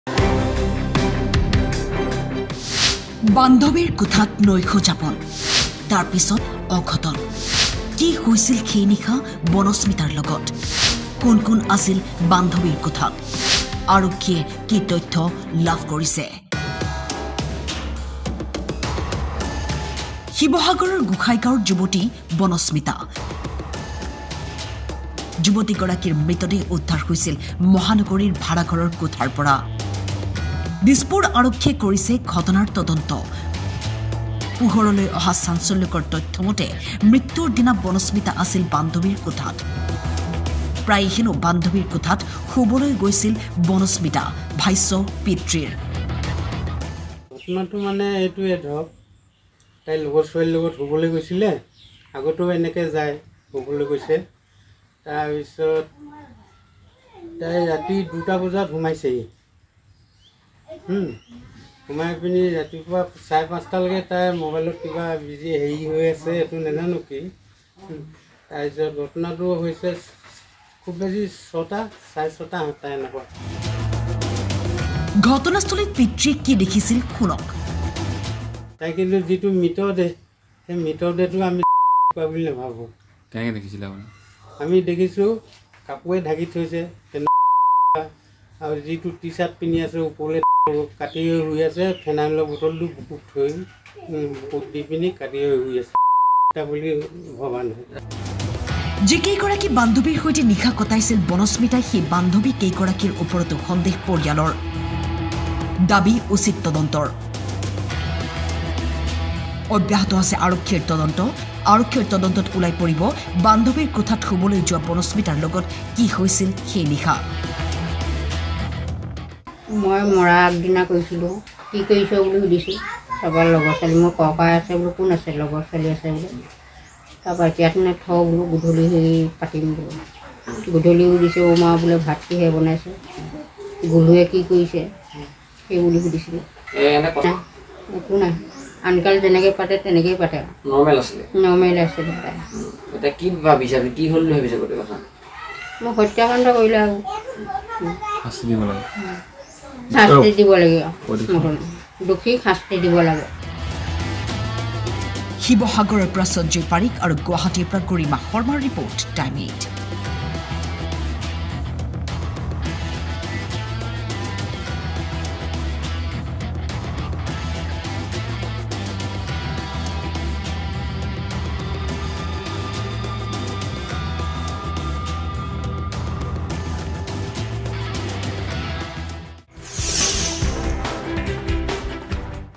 বনস্মৃতাৰ সৈতে কি হৈছিল সেই নিশা?
বান্ধৱীৰ কোঠাত নৈশ যাপন। তাৰ পিছত অঘটন! বনস্মৃতাৰ সৈতে কি হৈছিল সেই নিশা? আৰক্ষীয়ে কি তথ্য লাভ কৰিছে?